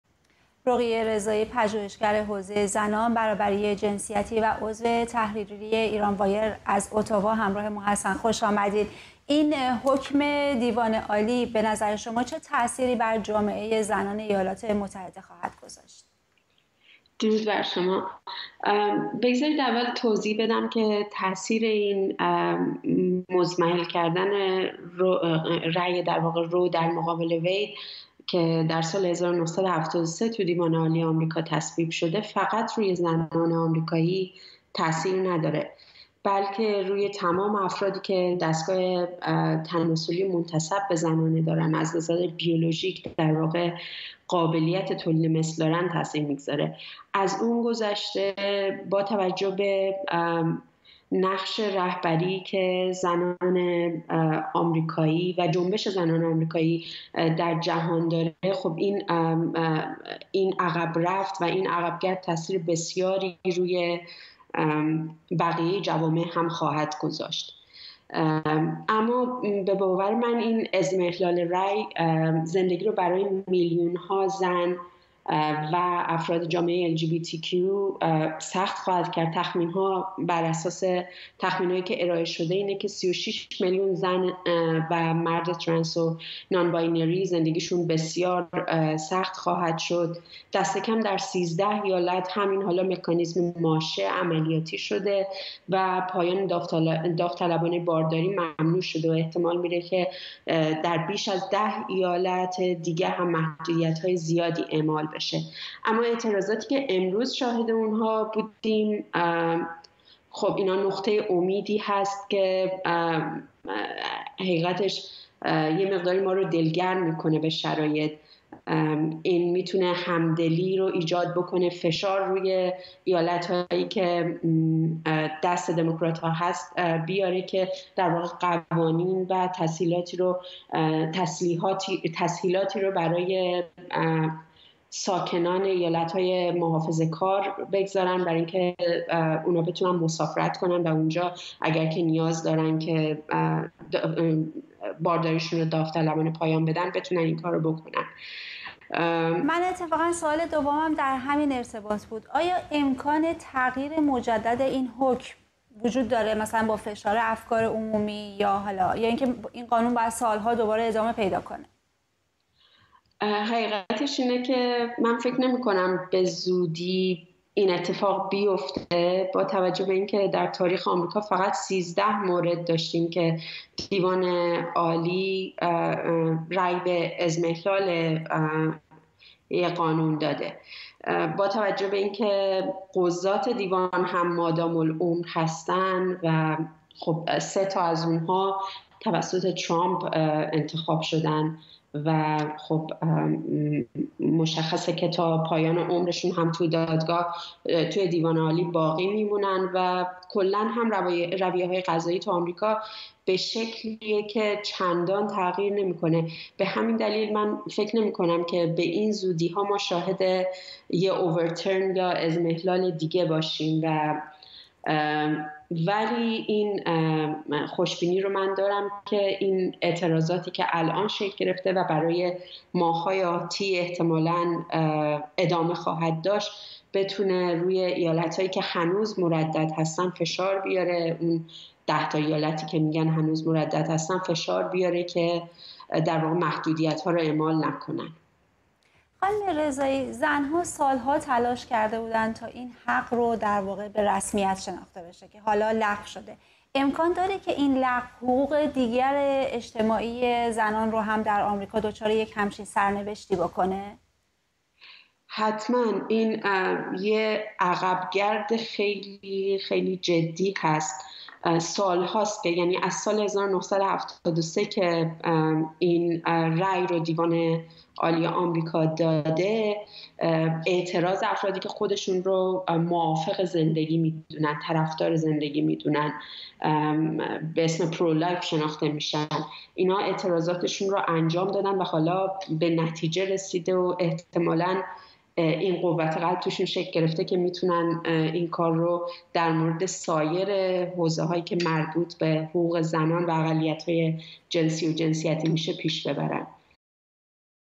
لغو قانون پایان خودخواسته بارداری در آمریکا بعد از ۱۵ هفتگی
رقیه رضایی، پژوهشگر حوزه زنان و برابری جنسیتی: حکم دیوان عالی آمریکا مبنی بر لغو پایان خودخواسته بارداری، نوعی عقب‌گرد در راستای احقاق حقوق زنان است که در آینده تاثیر بسیار زیادی بر روی جوامع دیگر هم خواهد گذاشت